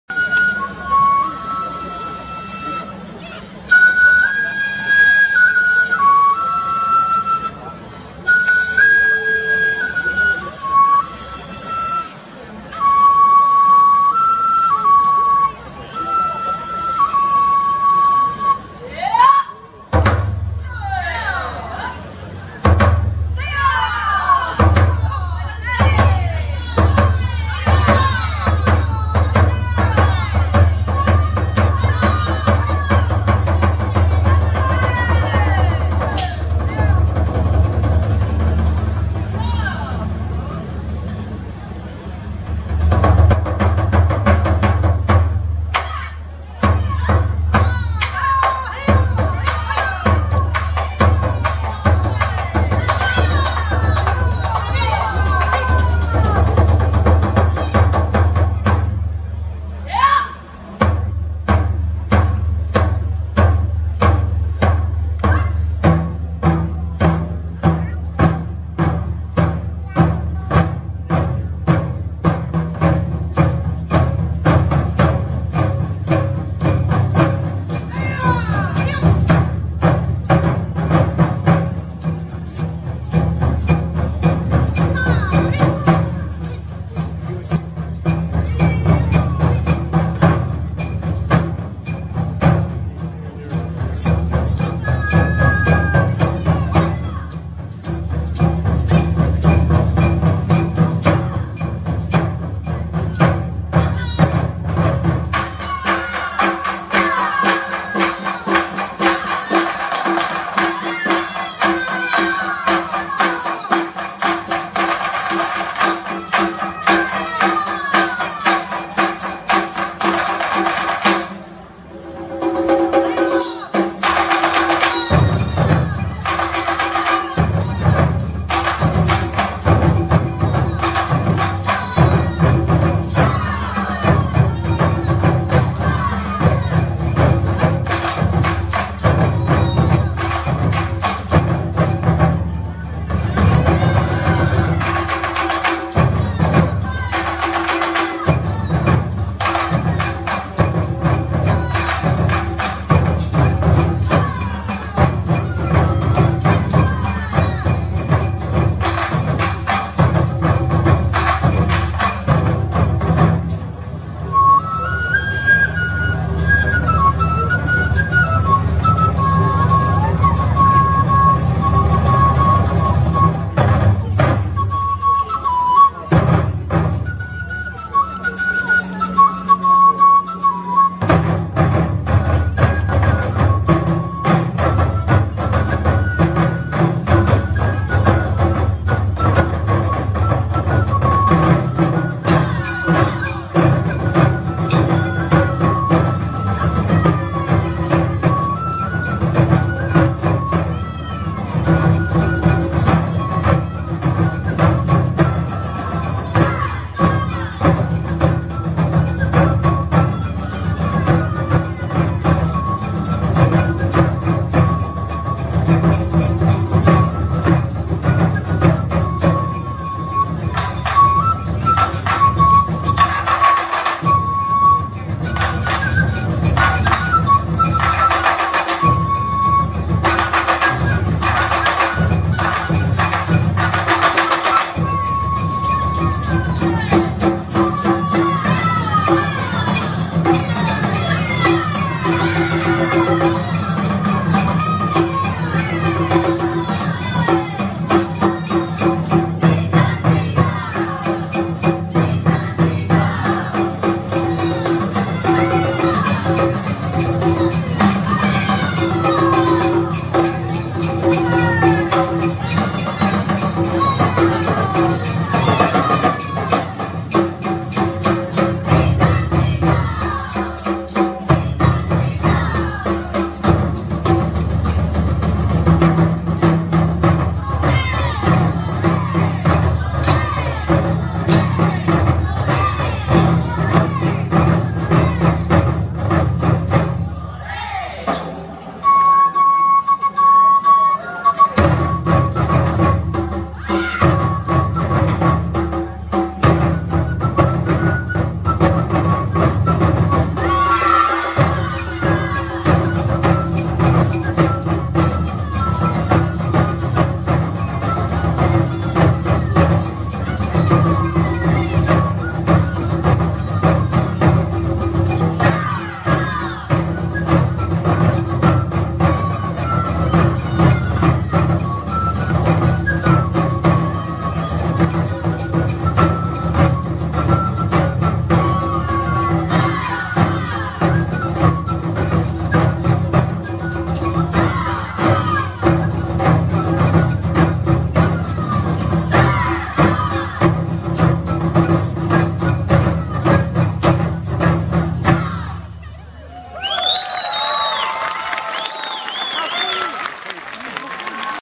Sawagi Taiko (さわぎ太鼓) @ 2017 Powell Street Festival, Vancouver, BC, Canada, Part II
Members of Sawagi Taiko (さわぎ太鼓) perform during the 41st Annual Powell Street Festival at Oppenheimer Park in Vancouver, BC, Canada on Saturday, Aug. 5, 2017.

Formed in 1990, Sawagi Taiko is the first all-Asian women taiko group in Canada. The group has performed at numerous music festivals and community events across Canada and the USA. Most of the pieces performed by Sawagi Taiko are composed or arranged by its members who, in addition to their taiko experience bring other artistic skills to the group, including movement and voice. With diverse experience as Asians, members of Sawagi Taiko explore their cultural roots through taiko. Their performances present an image of Asian women as strong and powerful.